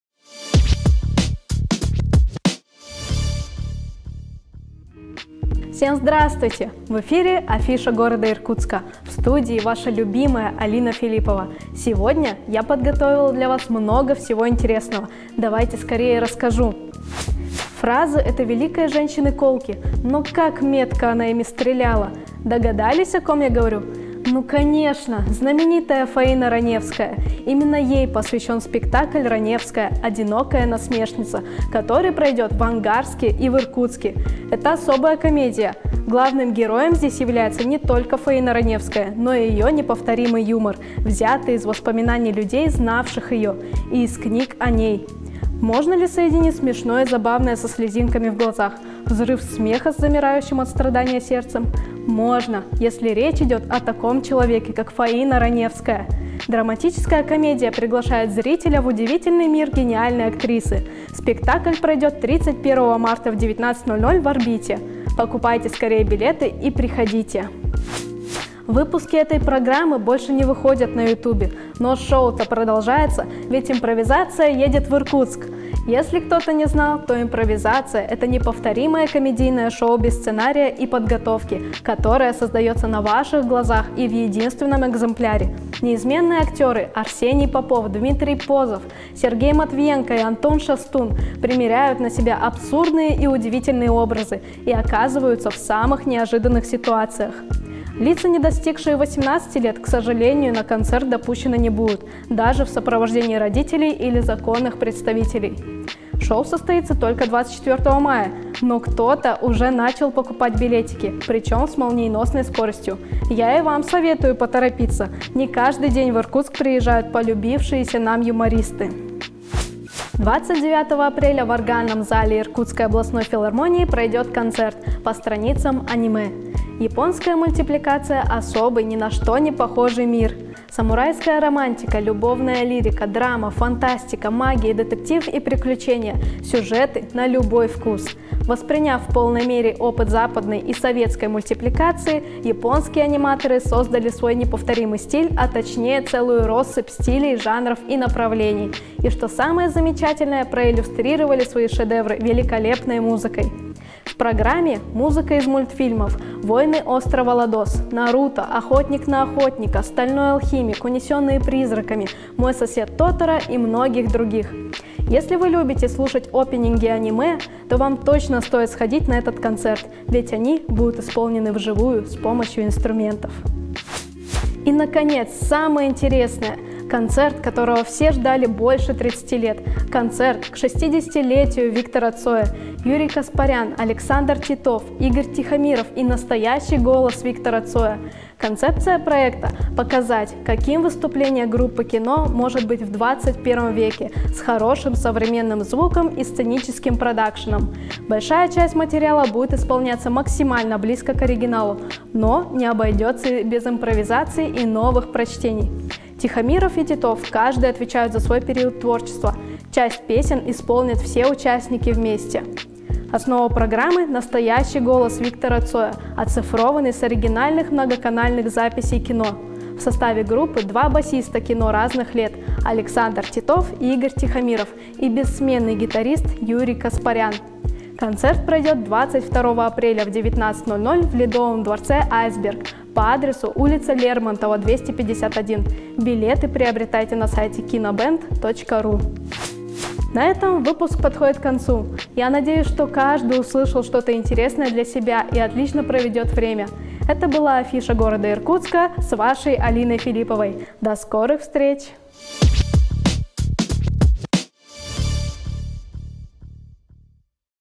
Афиша 23.03.2023
Спектакль, посвященный незабываемой Фаине Раневской, музыка аниме в Органном зале, шоу "Импровизация" и необычный концерт группы "Кино" с голосом Виктора Цоя - именно такой увидела предстоящую культурную жизнь Иркутска Алина Филиппова.